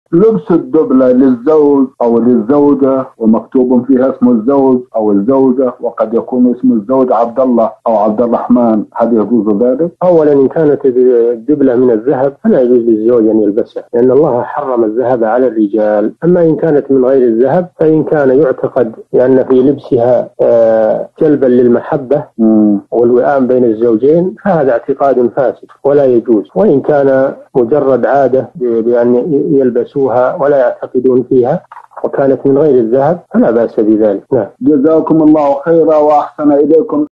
لبس الدبلة للزوج أو للزوجة ومكتوب فيها اسم الزوج أو الزوجة وقد يكون اسم الزوج عبد الله فهل يجوز ذلك؟
نص السؤال:
لبس الدبلة للزوج أو للزوجة ومكتوب فيها اسم الزوج أو الزوجة وقد يكون اسم الزوج عبد الله أو عبد الرحمن هل يجوز ذلك؟
السيرة الذاتية :
للاشتراك في قناة الشيخ صالح الفوزان

رئيس الفريق
لمشروع كبار العلماء